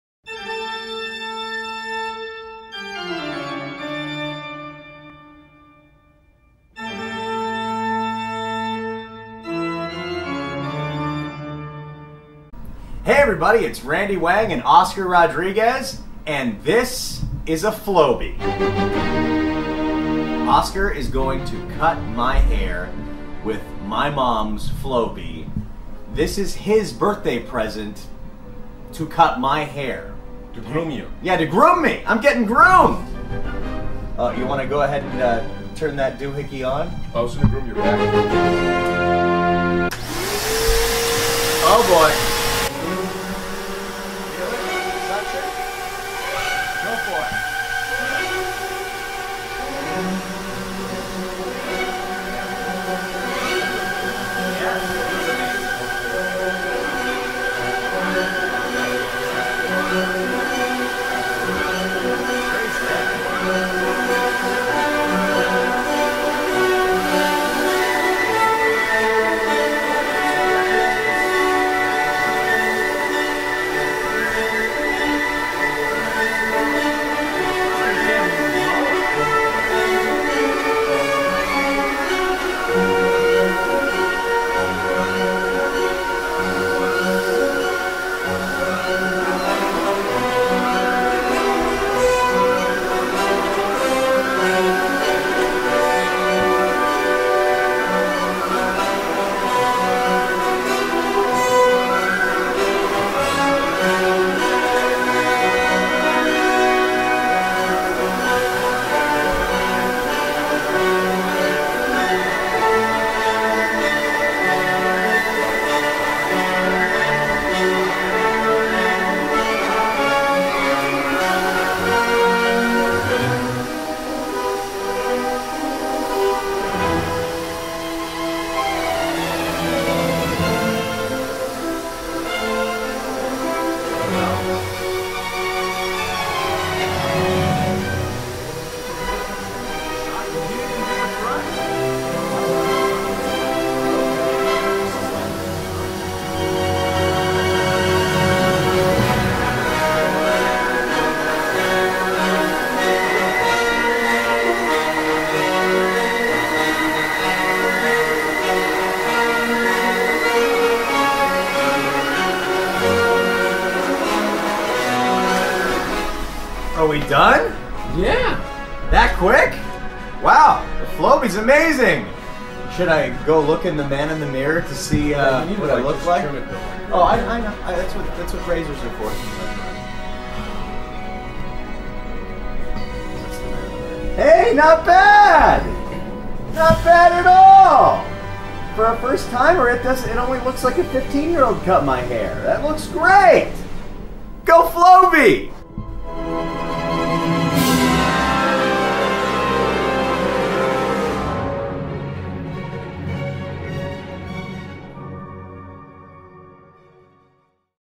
Go Flowbee!!!
For Oscar's birthday, Randy Wang has bestowed Oscar the honor of styling Randy's hair via Flowbee.